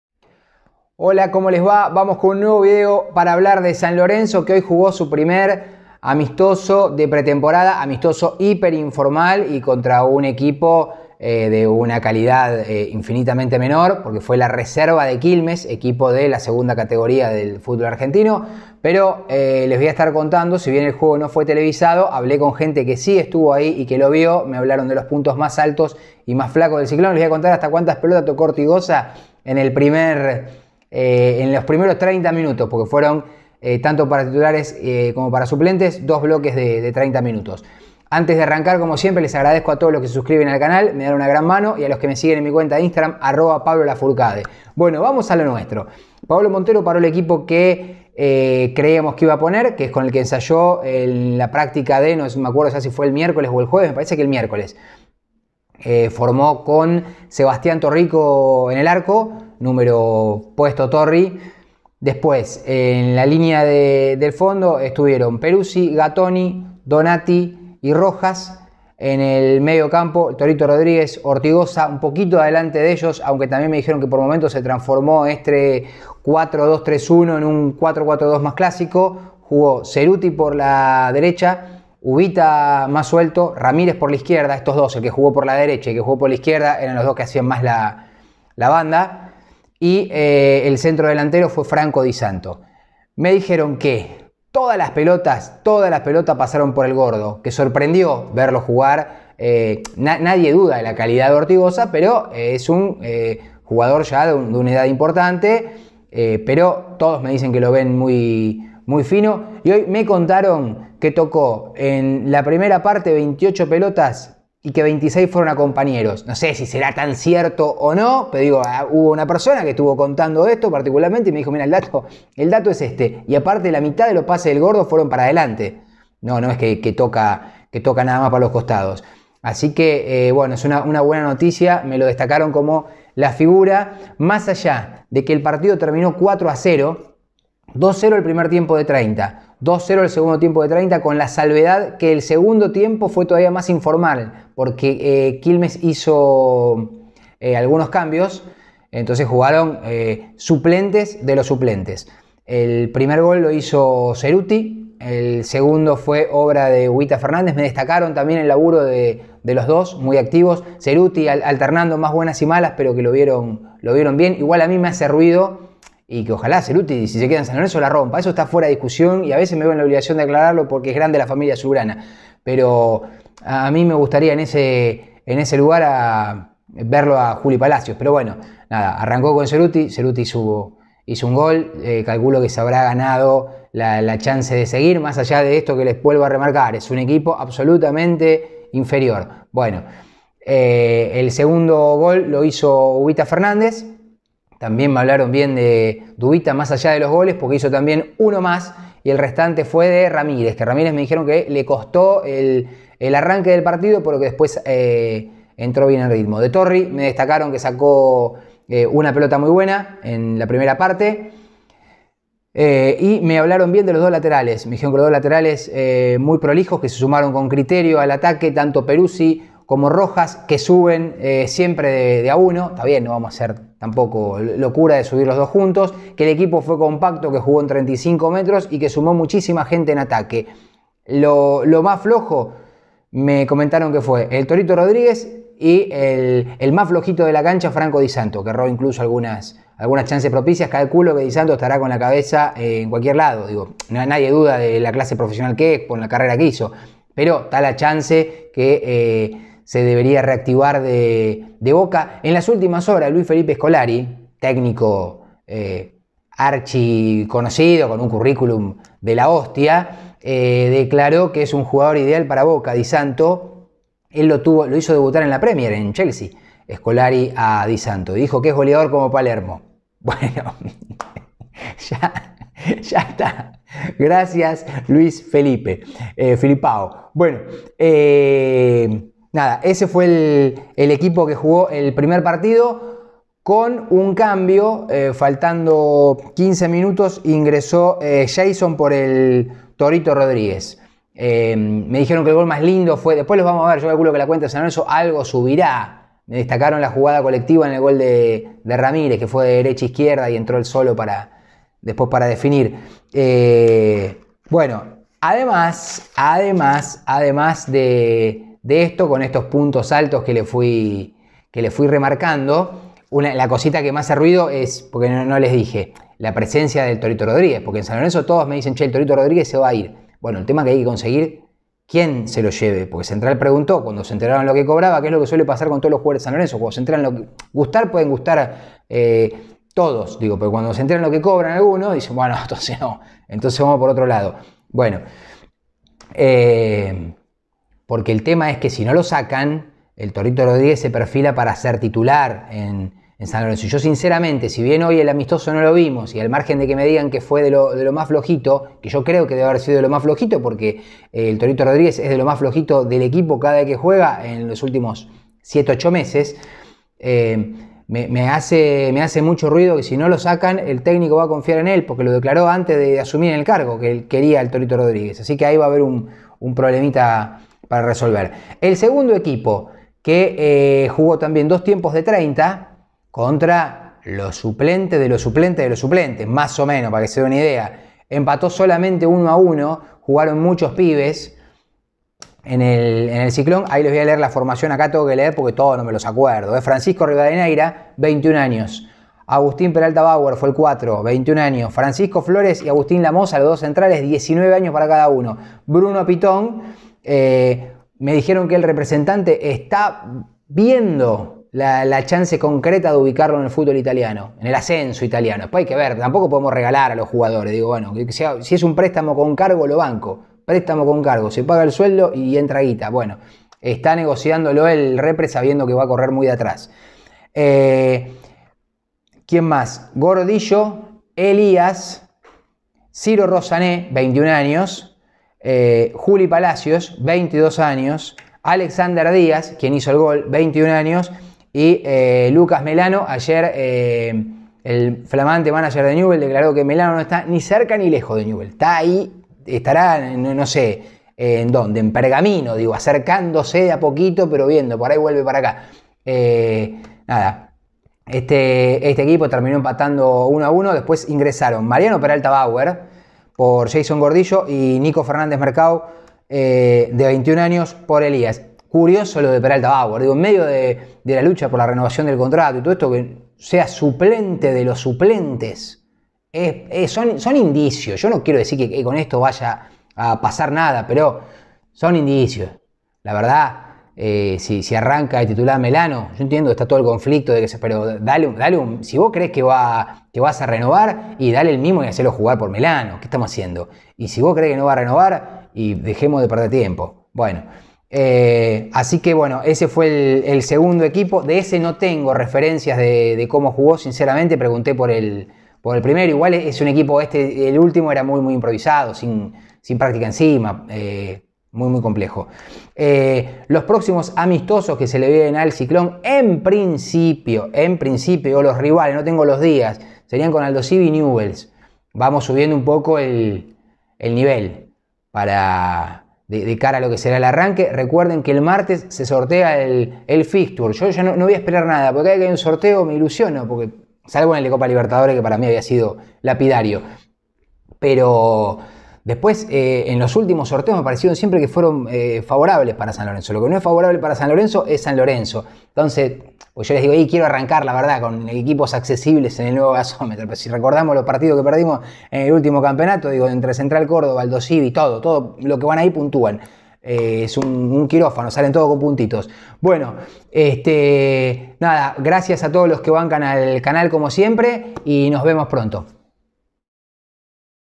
SAN LORENZO - TRIUNFO 4 A 0 SOBRE QUILMES EN EL PRIMER AMISTOSO DE MONTERO, CON ORTIGOZA CÓMO FIGURA
San Lorenzo le ganó 4-0 a los suplentes de Quilmes en un amistoso de 60 minutos en el Nuevo Gasómetro. El Ciclón de Paolo Montero formó con: Sebastián Torrico;  GinoPeruzzi, Federico Gattoni, Alejandro Donatti, Gabriel Rojas; Néstor Ortigoza, Diego Rodríguez; Ezequiel Cerutti, Nicolás Fernández, Juan Ramírez; Franco Di Santo.
El Pocho Cerutti, Uvita Fernández -2- y Ramírez, hicieron los goles.
Además, los suplentes empataron 1-1 con gol de Alexander Díaz.